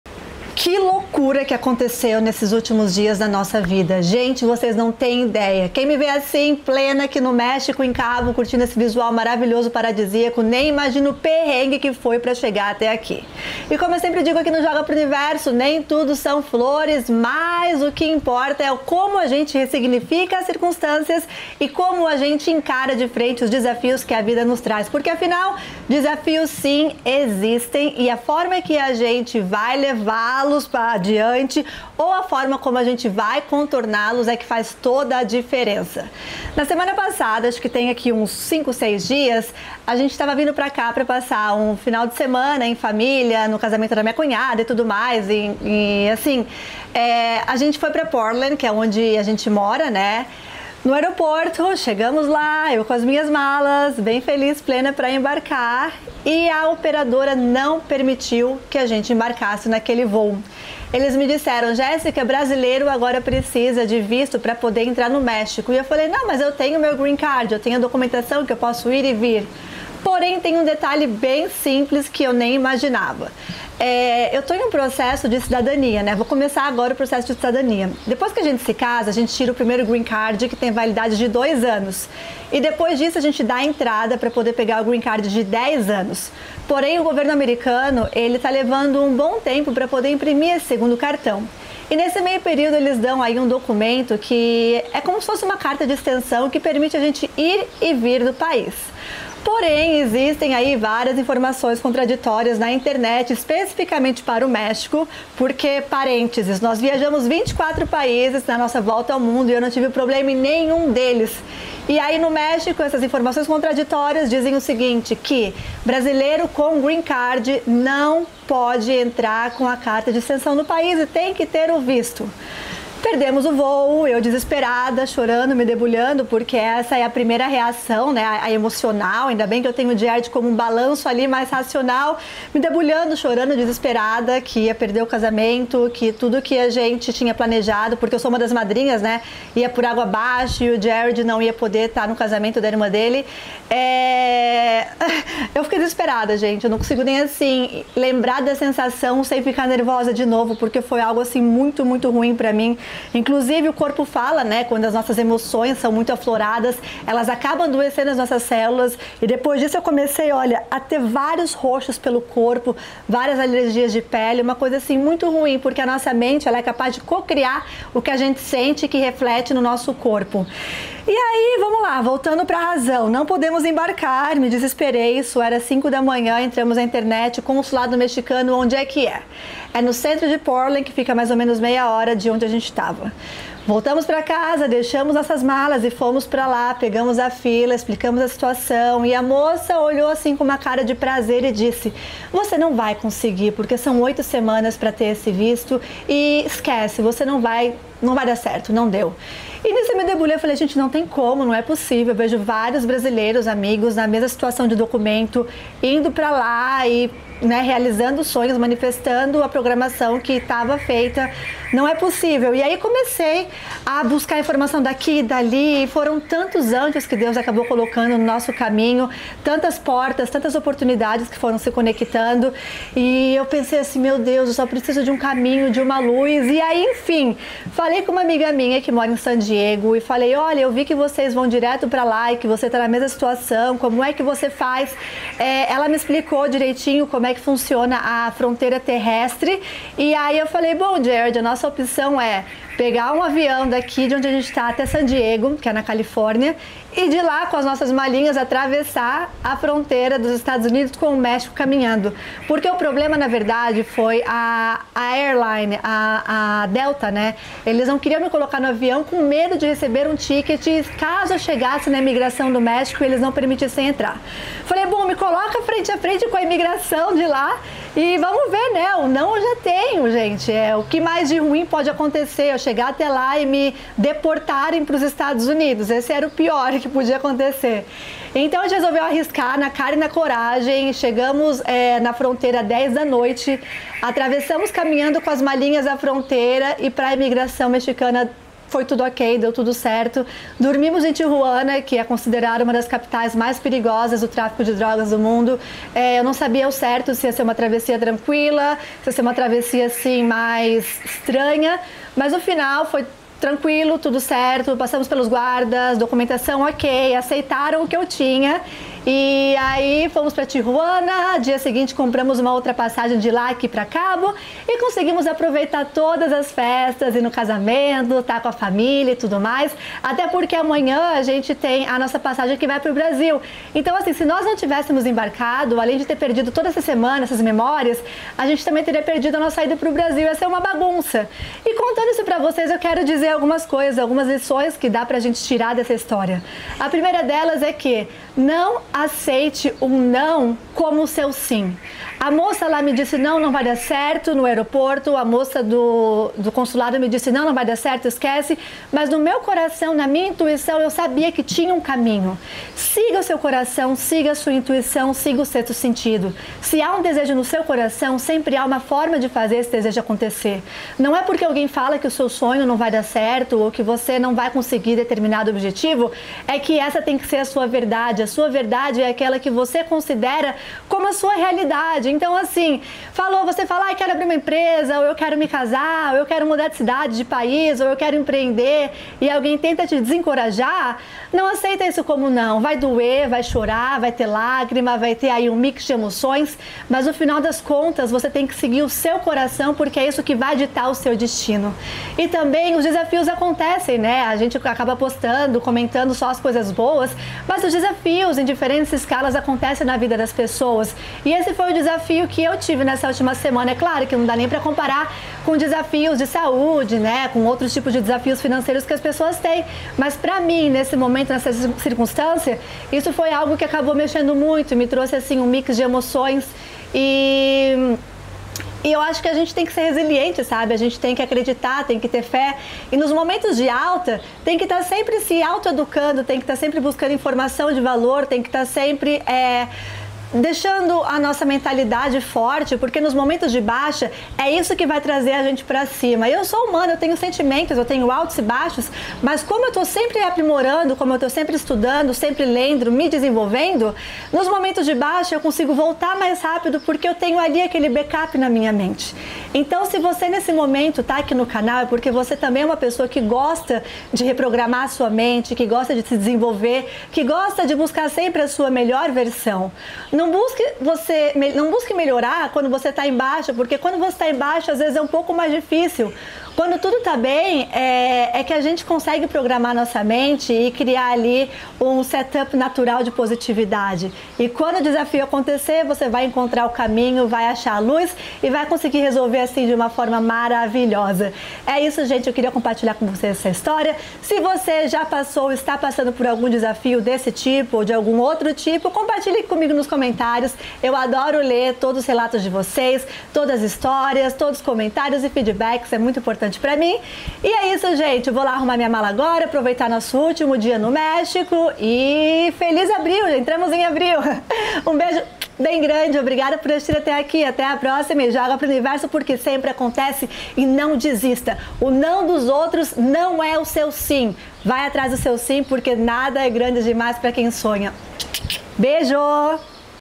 DESAFIO SUPERADO... Brasileira nos EUA Proibida de Entrar no México: Como Resolver Isso?
Nesse vídeo eu conto a experiência de ter sido proibida de embarcar para o Mexico saindo dos EUA, como resolvi isso, a mentalidade que me ajudou a superar esse desafio e as lições que tirei desse episódio.

Se você quer saber como entrar no México sem visto sendo brasileiro, ou se você está com a carta de extensão de seu green card e planeja voar para o México, assista esse vídeo.

✨ "Namastê, que a sorte que me acompanha também acompanhe você".

Joga Pro Universo 🙌🏼🌍🌟
Jéssica Denz